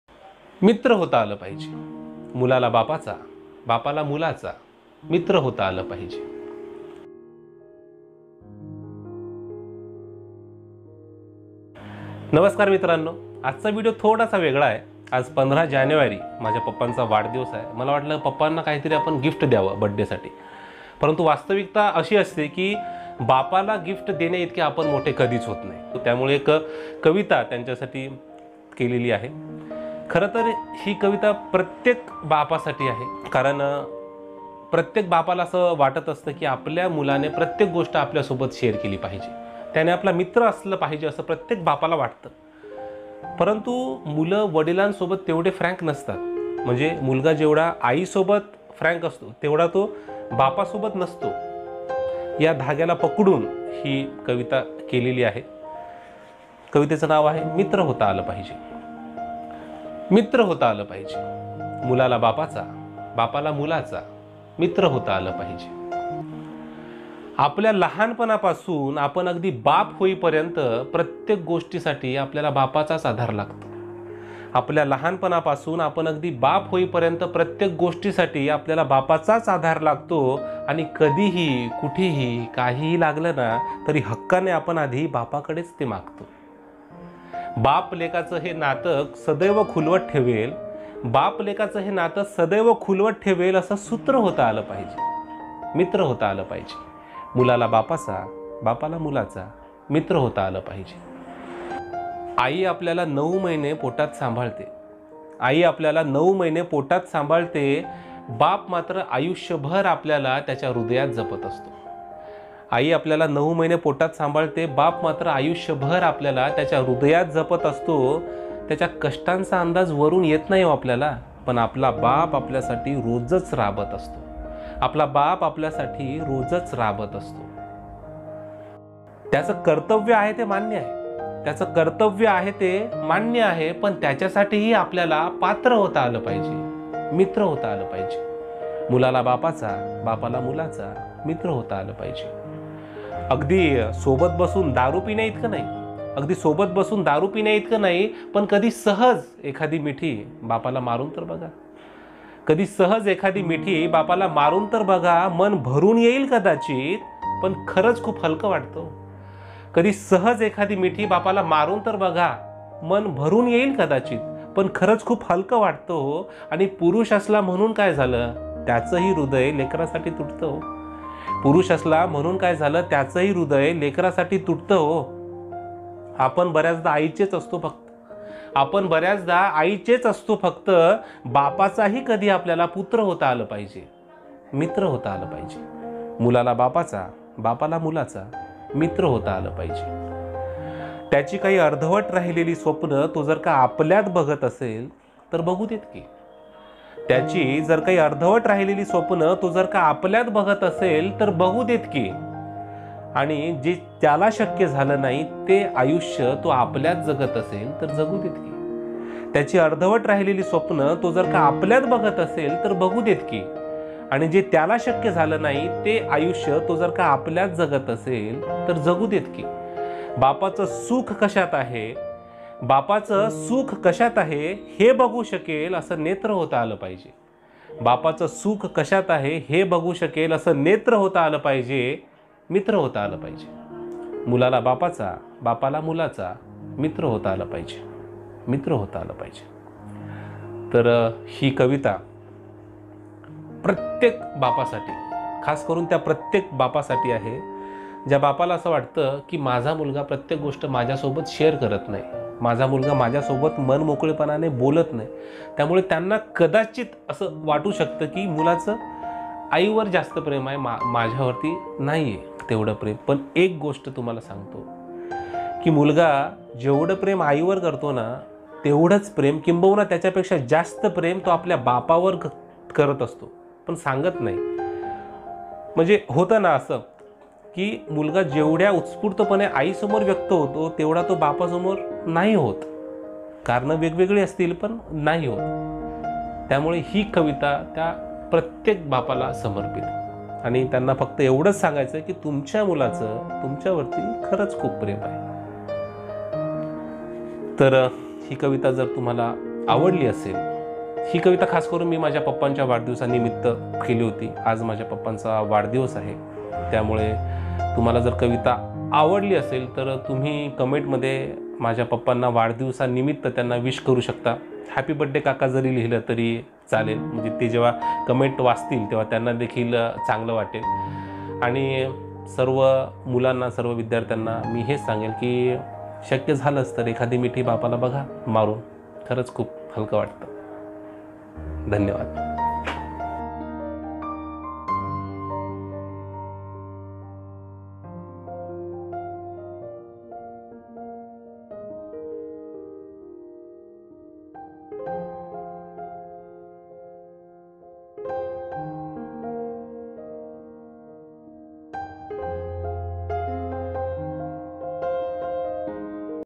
मित्र होता आलं पाहिजे.. मुलाला बापाचा , बापाला मुलाचा..| Marathi Kavita On Father | Jivan Aghav
कवी : जीवन सदाशिवराव आघाव

ही कविता त्या प्रत्येक बापासाठी आहे ज्या बापाला असं वाटतं की आपला मुलगा आपल्या सोबत तेवढं मनमोकळेपणाने बोलत नाही.
त्या प्रत्येक बापाला मला हेच सांगायचंय की तुमच्या मुलाचं तुमच्यावर खरंच खूप प्रेम आहे. तो व्यक्त होत नसेल कदाचित तुमच्या समोर पण म्हणून त्याचं प्रेम कमी होत नाही.